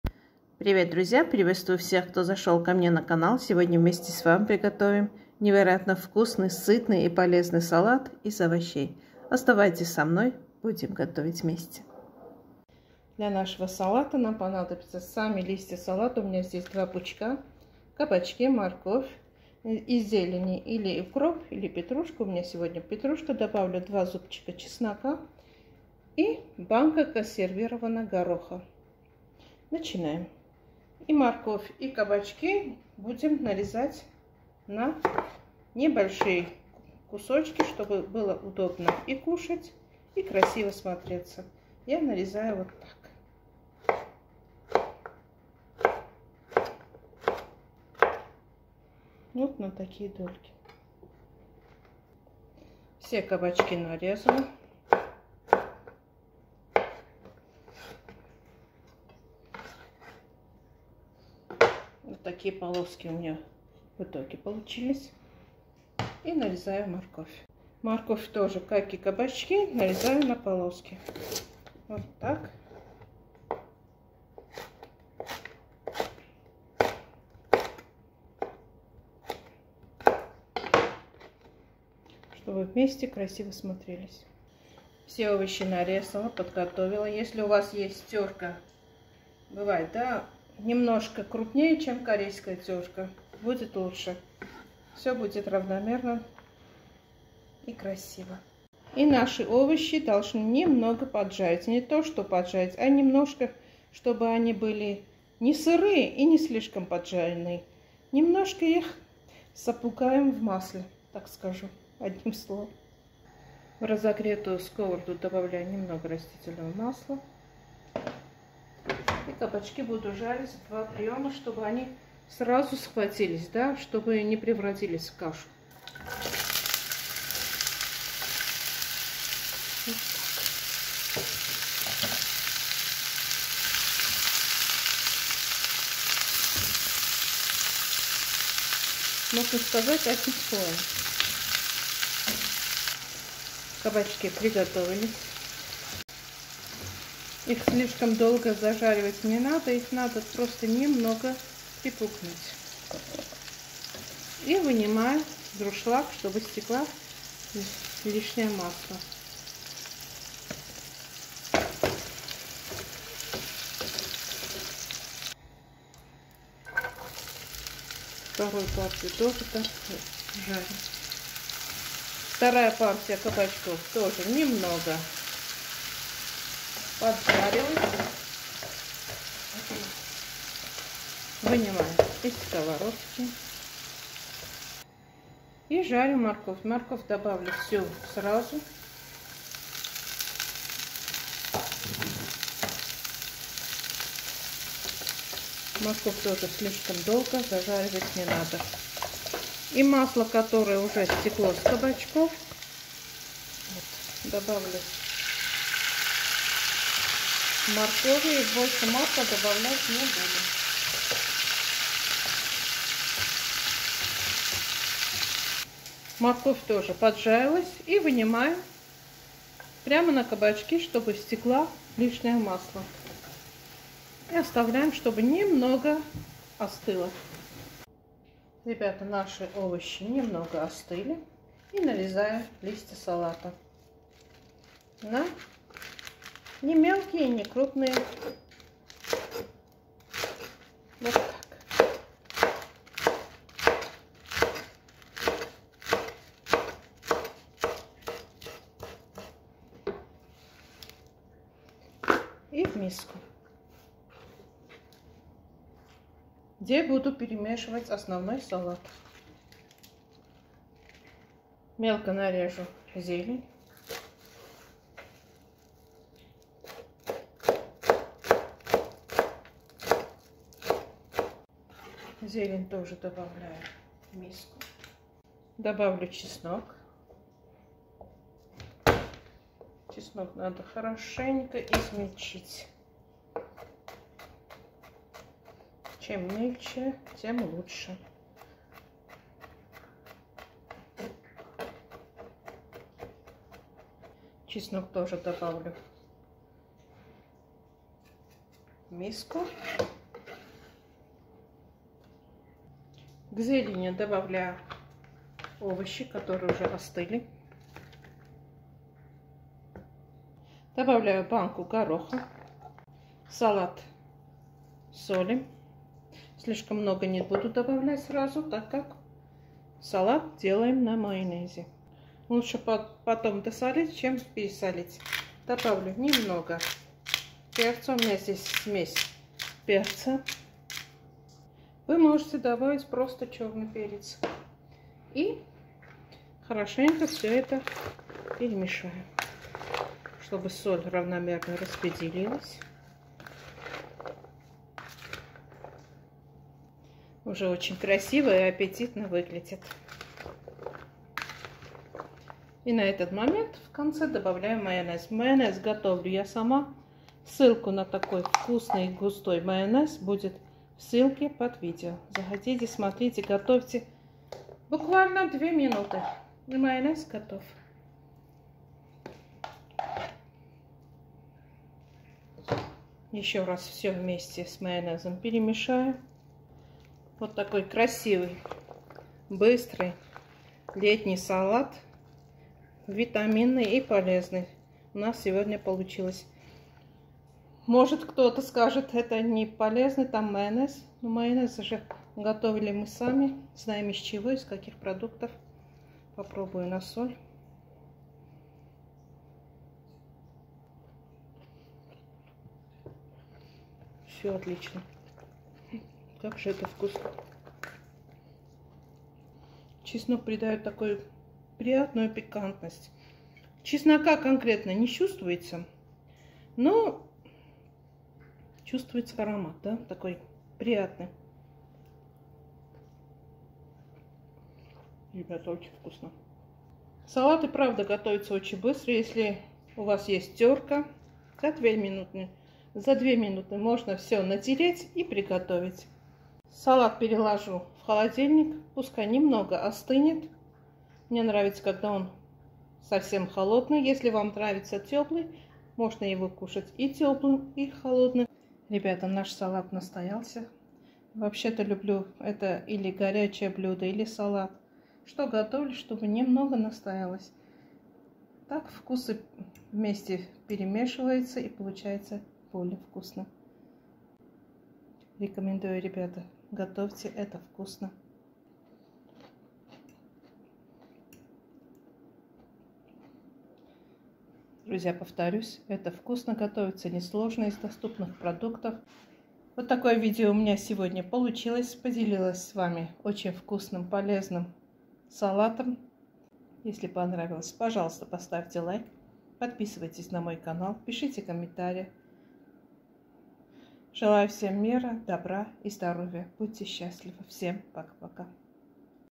Салат с Кабачками /Ну Очень Вкусный /для любителей кабачков 👍
Горох консервированный  400 гр
морковь  500 гр
кабачки  900 гр
листья салата  300 гр
зелень , чёрный перец  , соль
майонез,  чеснок